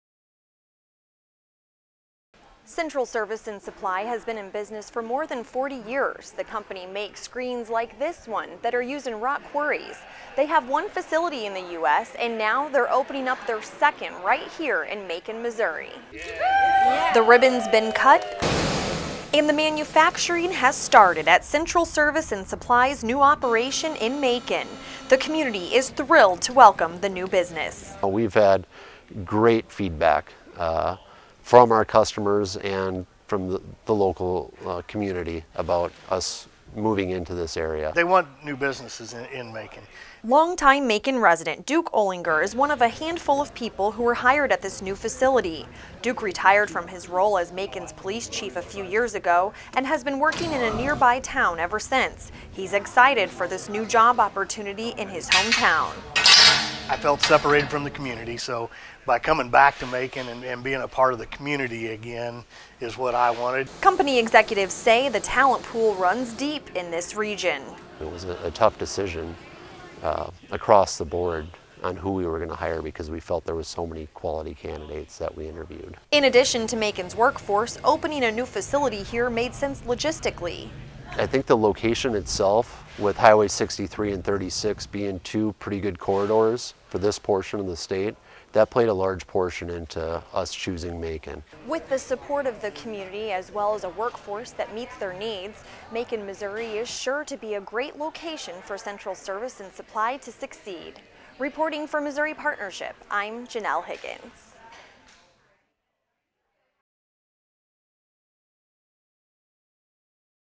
Missouri Selected For Advanced Manufacturer's New Facility
Central Service & Supply, Inc., an advanced manufacturer headquartered in Des Moines, Iowa, recently opened its second U.S. facility in Macon, Missouri. Company executives say access to talent and logistical advantages are the main reasons they selected Missouri.